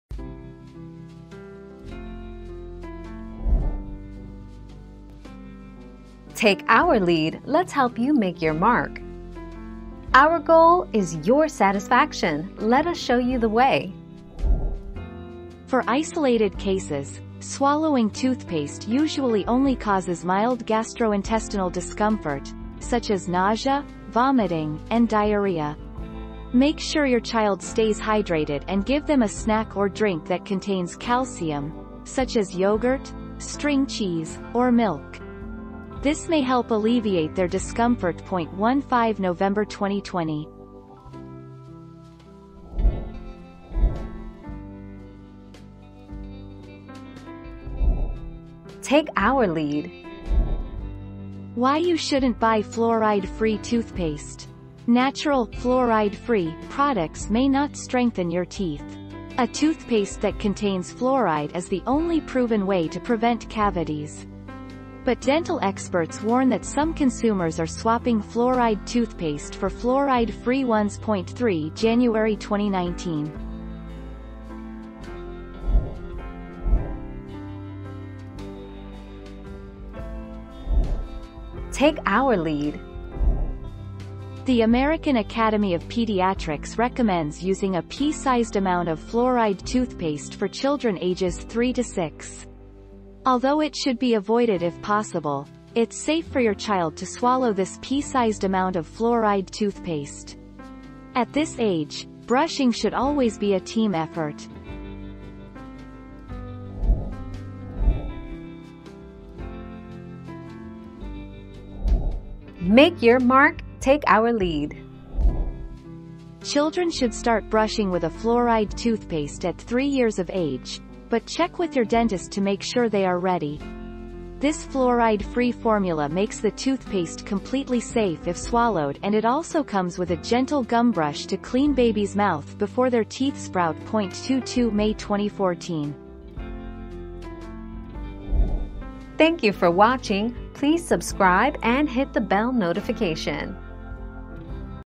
What happens if child swallows toothpaste?
00:00 - What happens if child swallows toothpaste?
00:44 - Are fluoride-free toothpaste good?
01:17 - Is children's toothpaste safe to swallow?
01:48 - Is fluoride-free toothpaste safe to swallow?

Laura S. Harris (2021, April 14.) What happens if child swallows toothpaste?
    AskAbout.video/articles/What-happens-if-child-swallows-toothpaste-236978